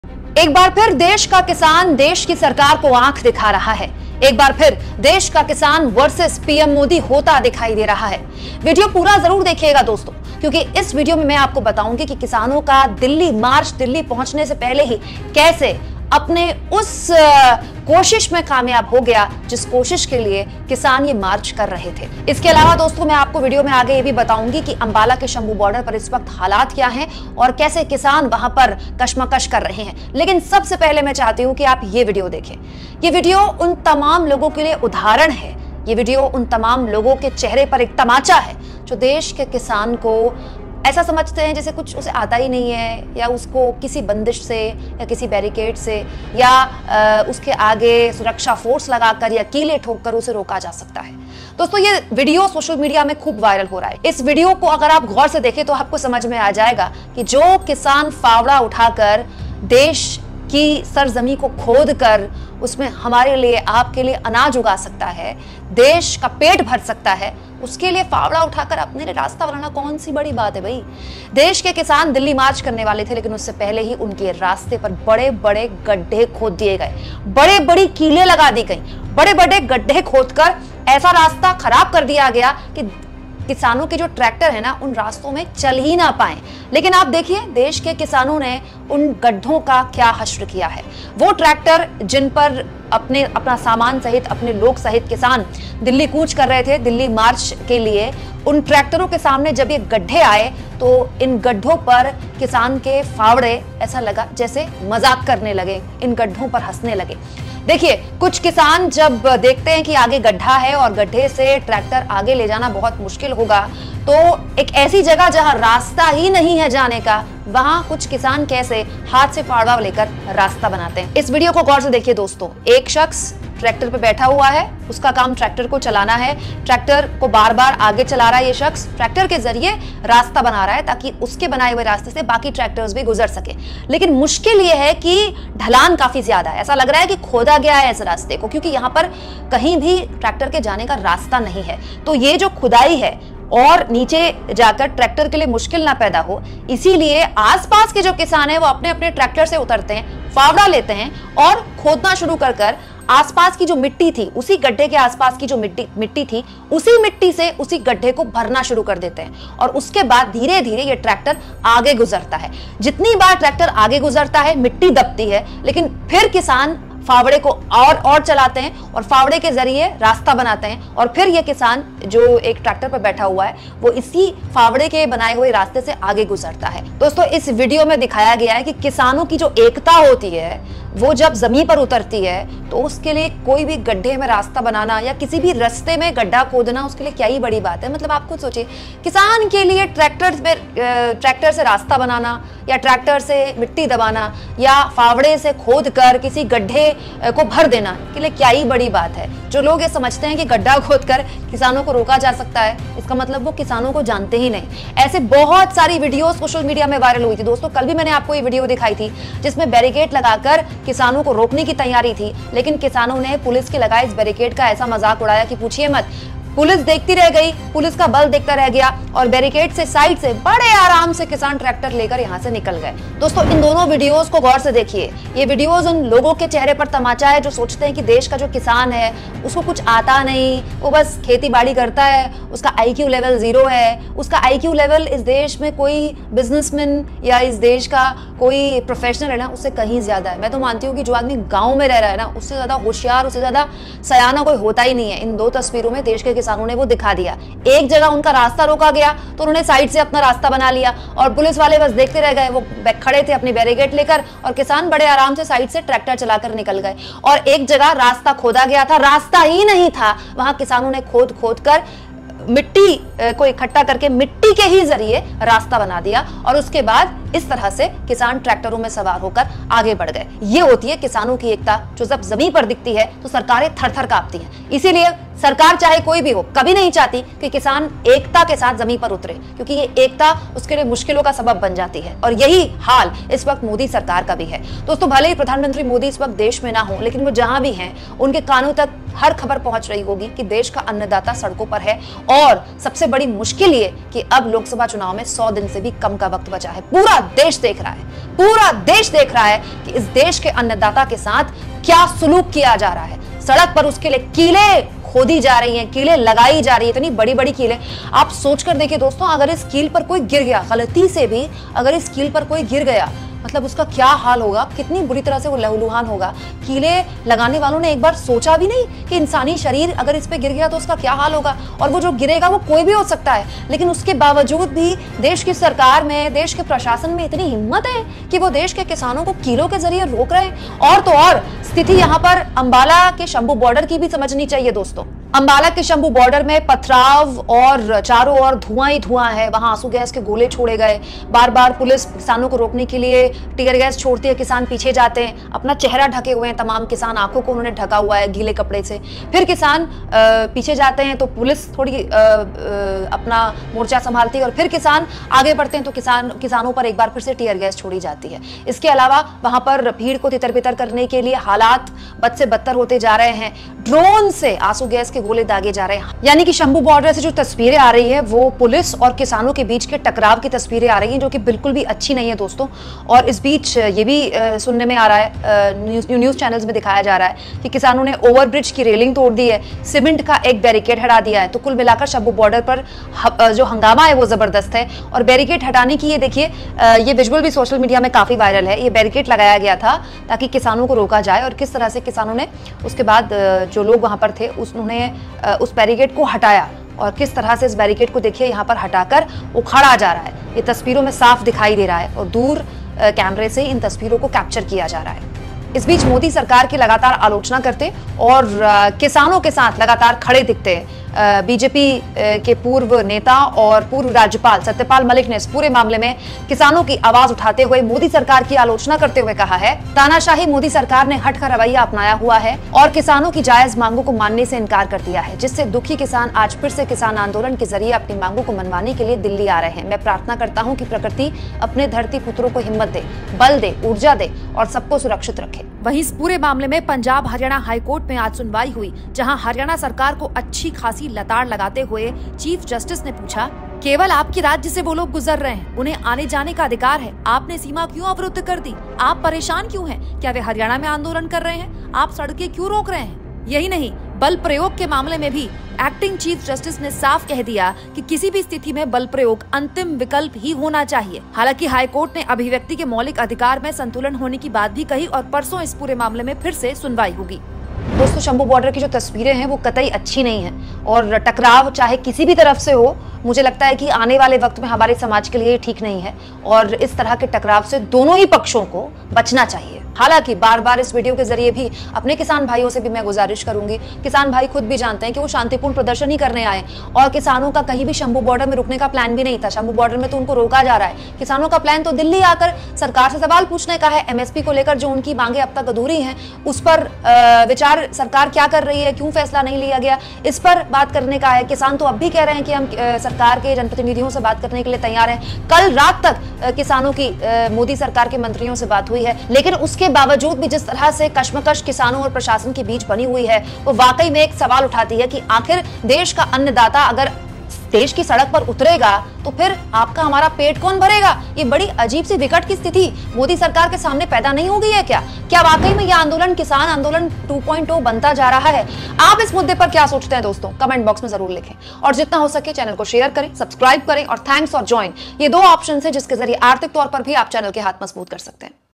Satya Pal Malik ने Modi को ललकारा! Court ने फटकारा, फंसी Sarkar? Farmers Protest | Kisan Andolan
The Punjab and Haryana High Court on Tuesday issued notice to the Central government and the States of Punjab, Haryana and Delhi on petitions related to the ongoing farmers' protest march to the national capital. At the same time, former BJP governor Satyapal Malik also supported the farmers while criticizing the Modi government. Kumkum Binwal analysis.